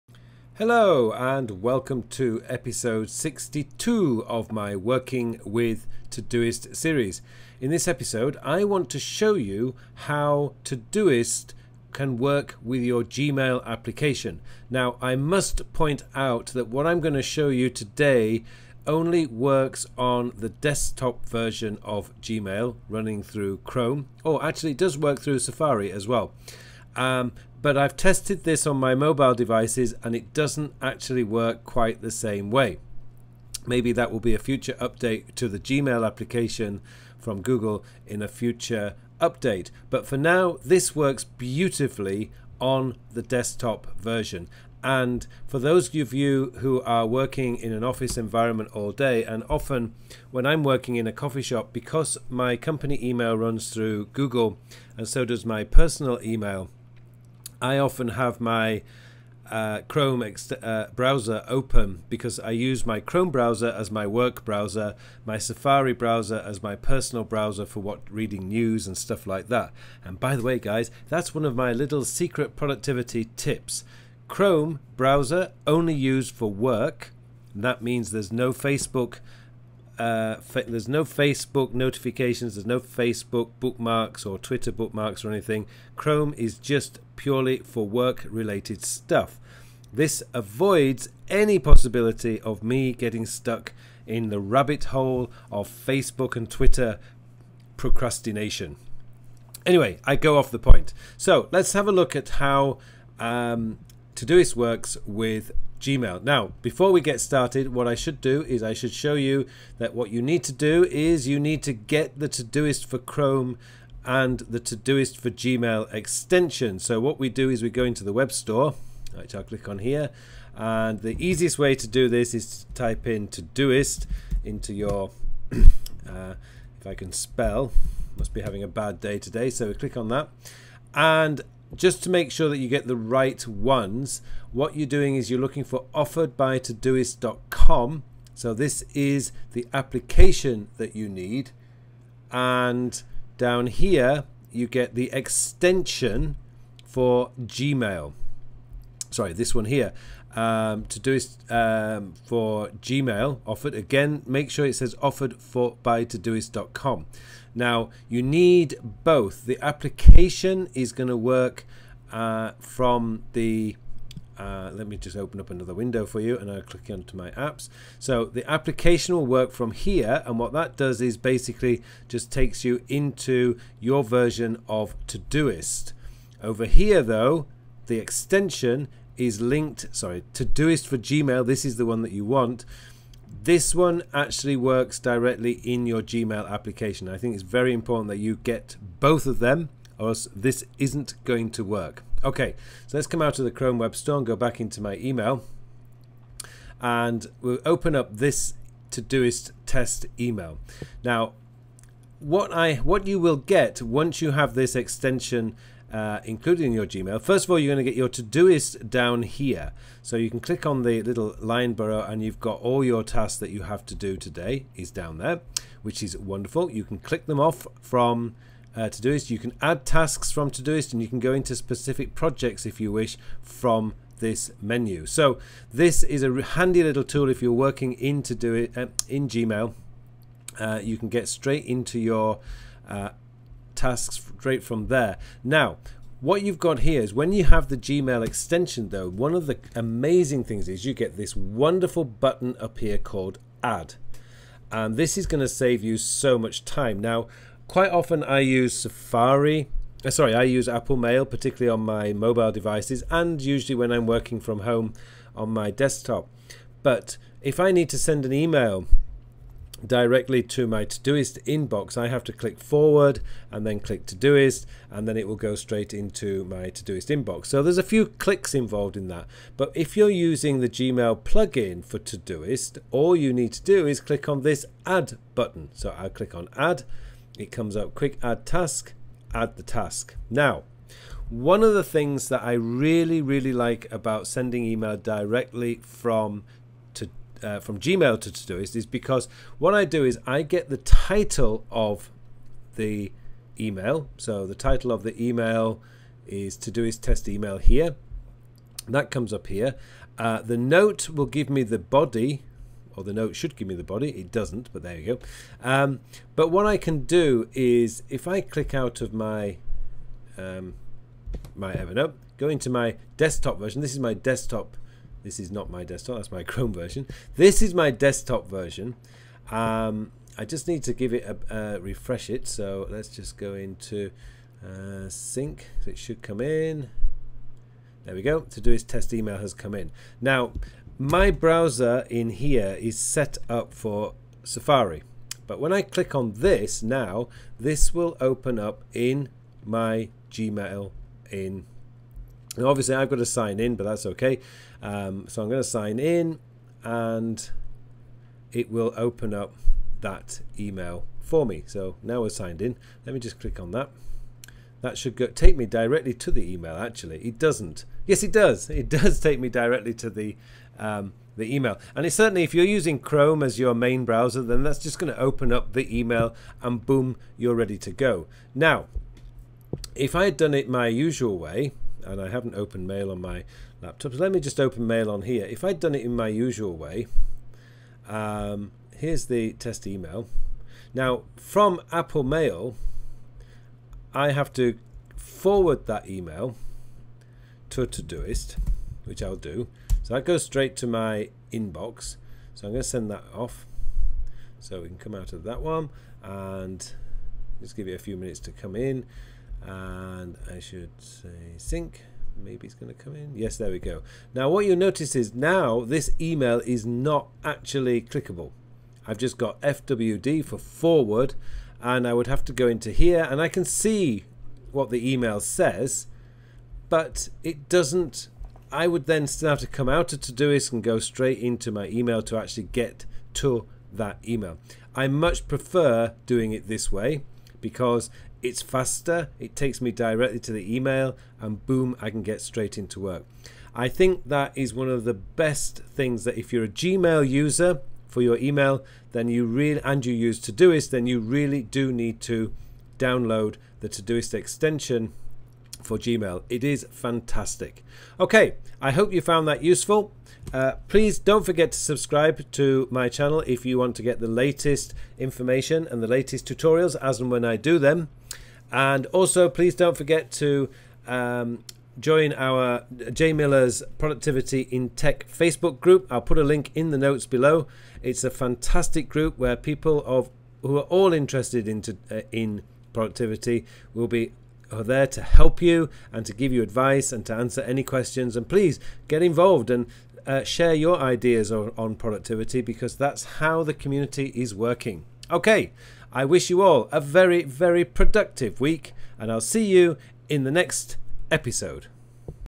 Working With Todoist | Ep 62 | Todoist & Gmail
This episode takes you through the steps to set up Todoist to work seamlessly with Gmail. A superb little extension for your Chrome browser.

*** LINKS ***